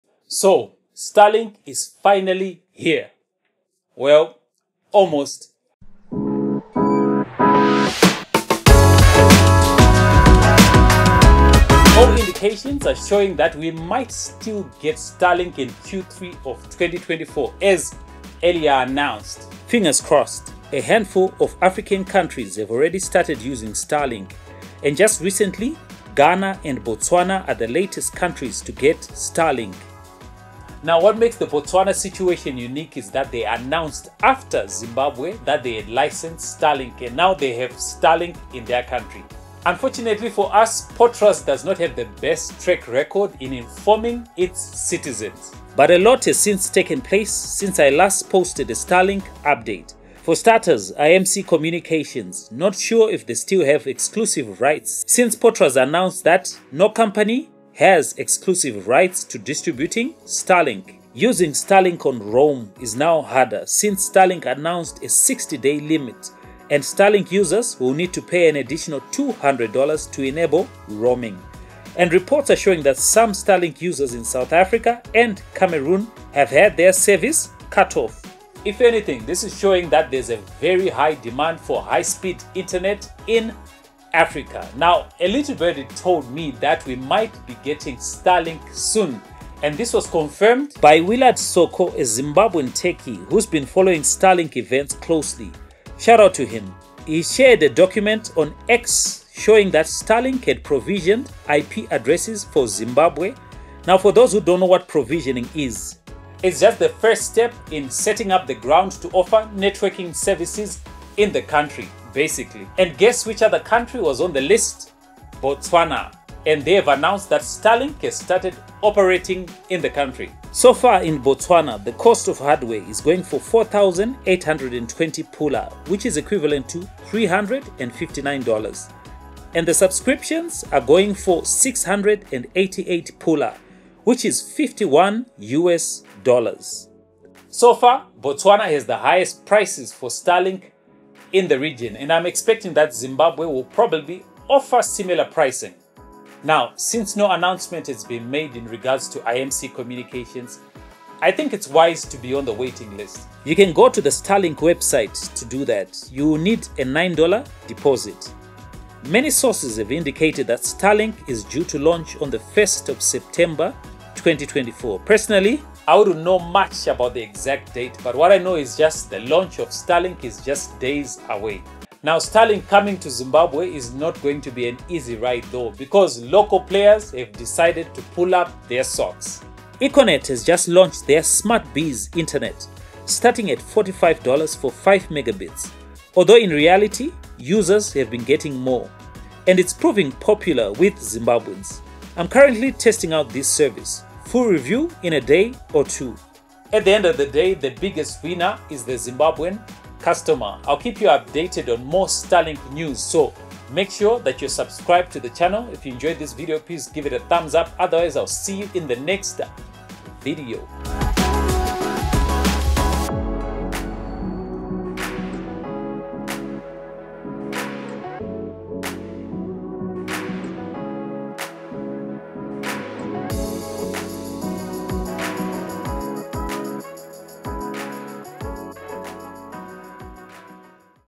Starlink Finally launches In Zimbabwe?
In this video i give an update on the imminent launch of Elon Musks Starlink internet in Zimbabwe.

If you find my content useful or helpful in any way please consider supporting the channel, using any of the methods below